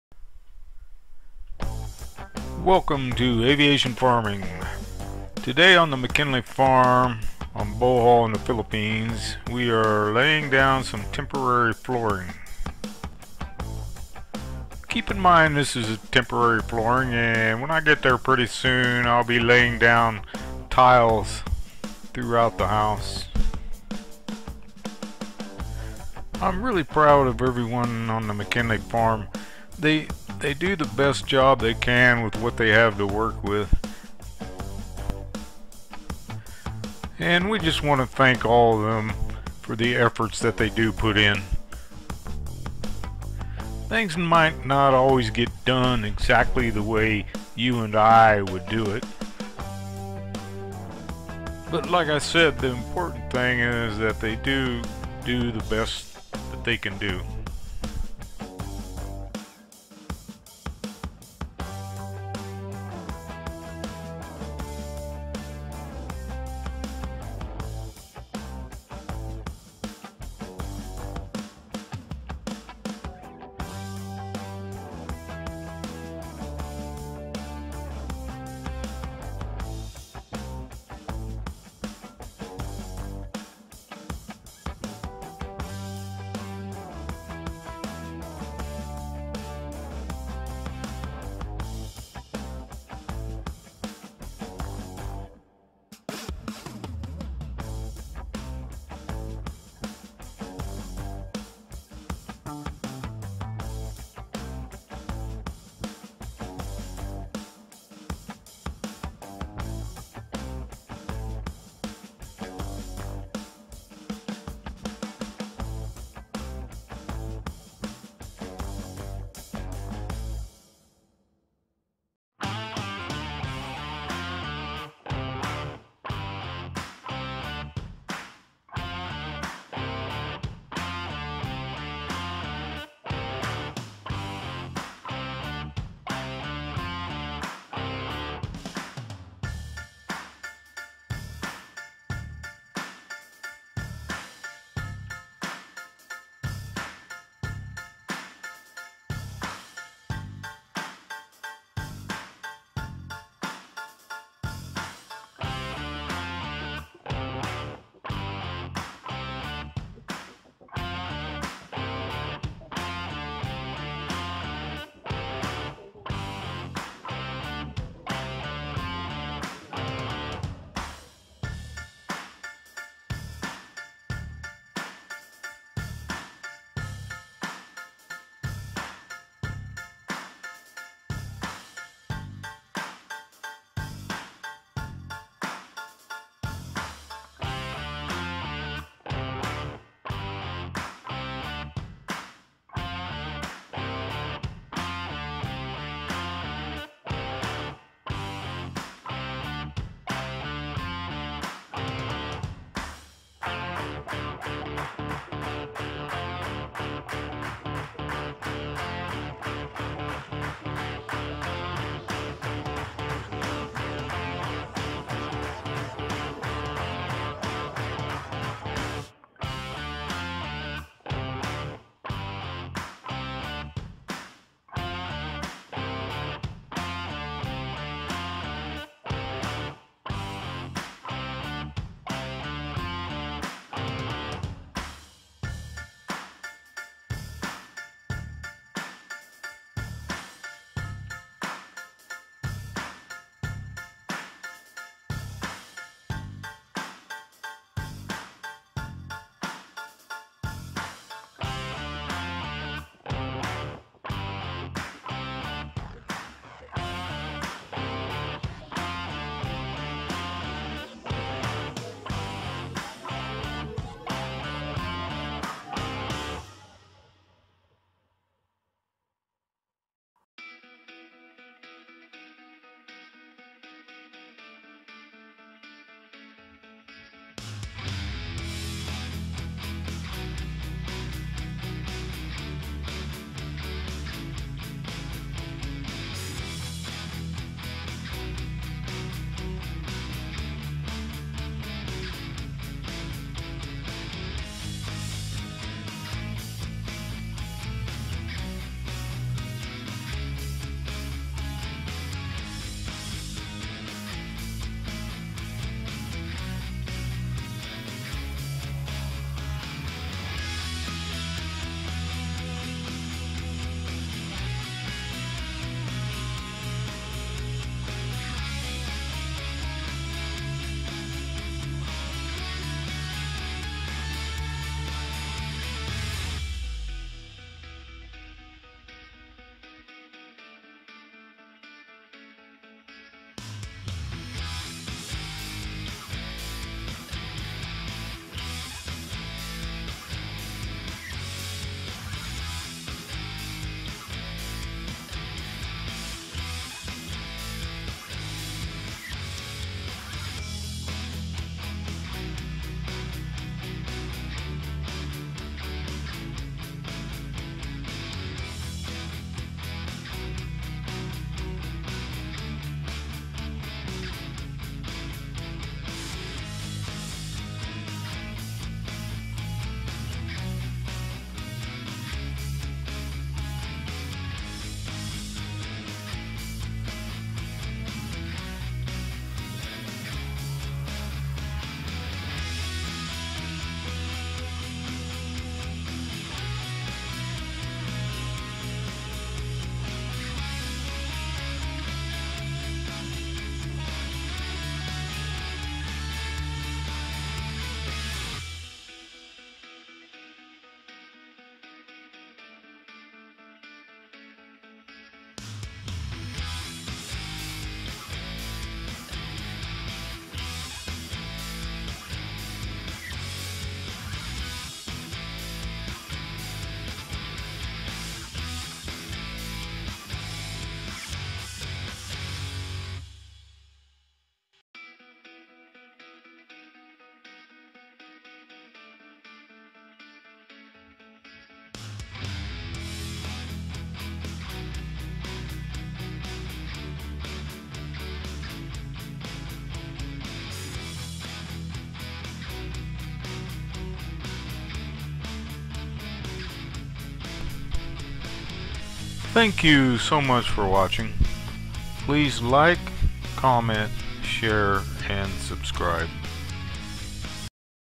Laying Down Temporary Flooring
Today on the Mckinley Farm temporary flooring was rolled out until I will be there and tile the floor. Everyone on the Mckinley Farm does the best job they can with what they have to work with. We are proud of them and their work for that. They might not do the work like you, or I would do it, but they do their best. Aviation farming, Mckinley Farm, Bohol, Philippines. Organic farming.